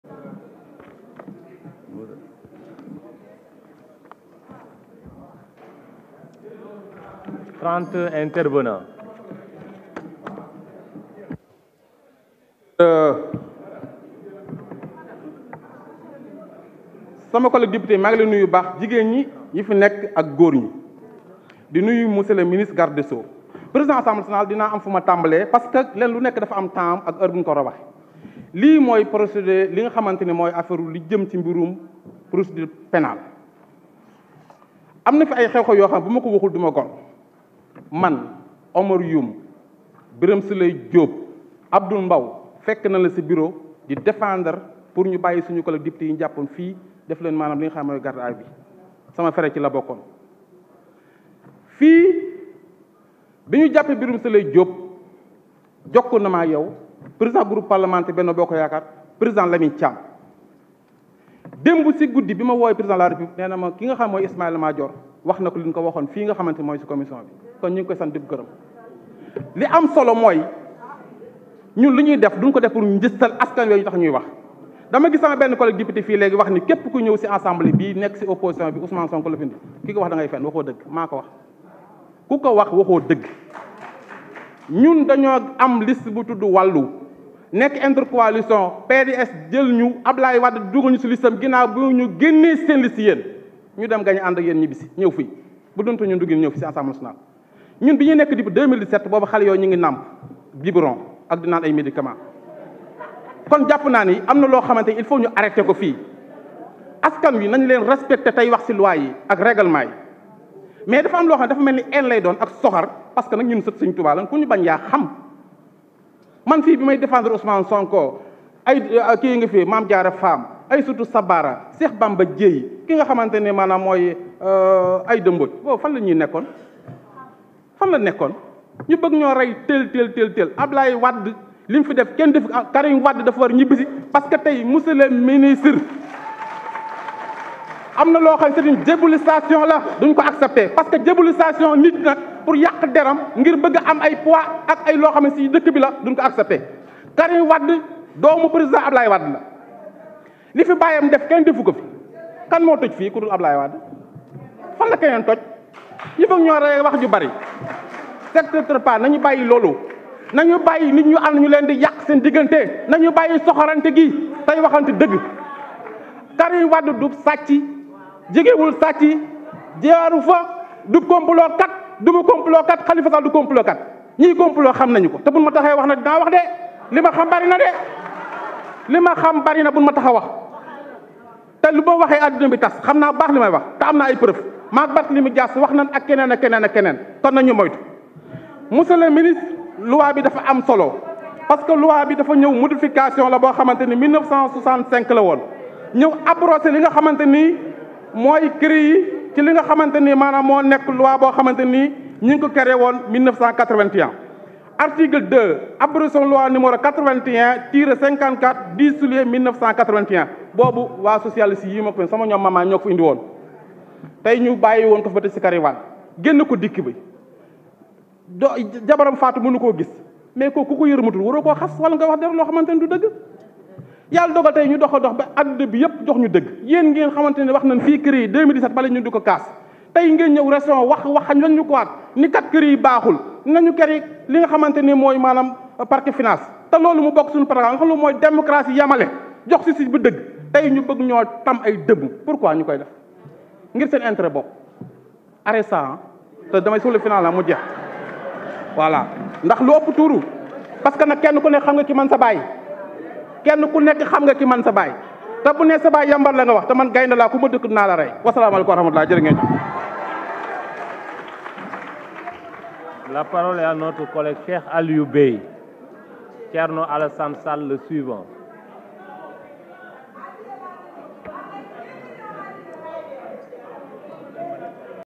Cheikh Bara Dolly aux députés de Pastef: « Kou wakh daniou dialogue-deal, da ngay fen »
1ère session extraordinare 2023: Le minister de la justice face aux députés